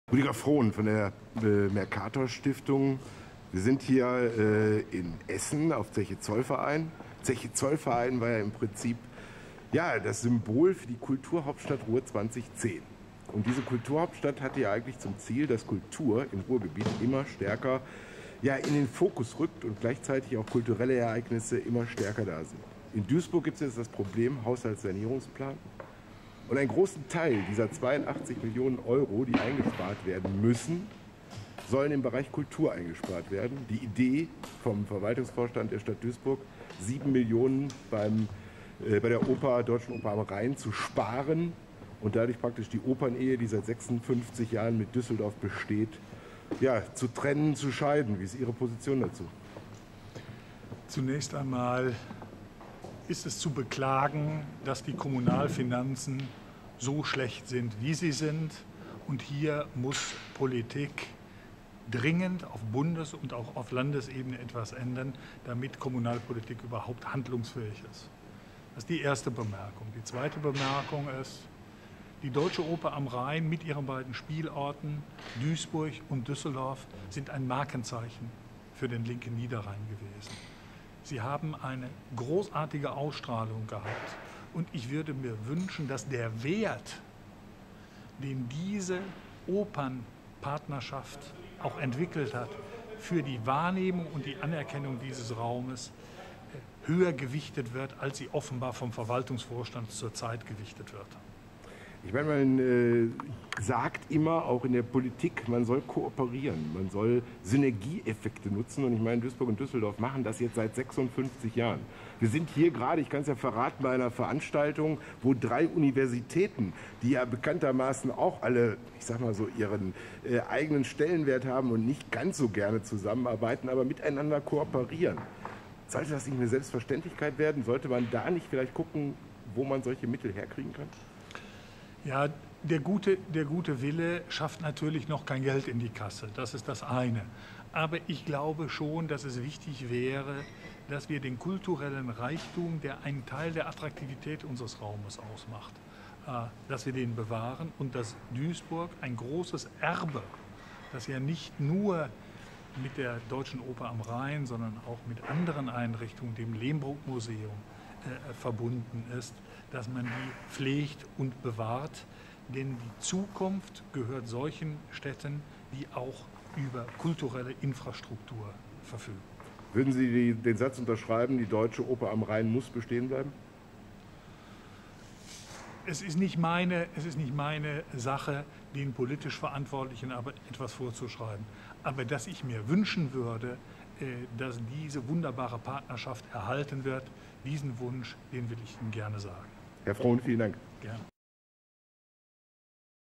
Rüdiger Frohn, Mercatorstiftung, zu Duisburger Plänen Opernvertrag mit Düsseldorf zu kündigen
Am Rande einer Veranstaltung auf Zeche Zollverein in Essen hatten wir gestern, Mittwoch, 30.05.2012 die Gelegenheit mit Rüdiger Frohn über die Pläne des Verwaltungsvorstandes der Stadt Duisburg zu sprechen, den Opernvertrag mit Düsseldorf zu kündigen, was ein Aus für die Deutsche Oper am Rhein bdedeuten würde. Hören Sie in dem folgenden Videogespräch, was der Vorsitzende des Beirates der Mercatorstiftung, der ehemalige Staatssekretär und Leiter des Bundespräsidialamtes Rüdiger Frohn zu sagen hat.
Gespräch und Redaktion: Frank-M. Fischer,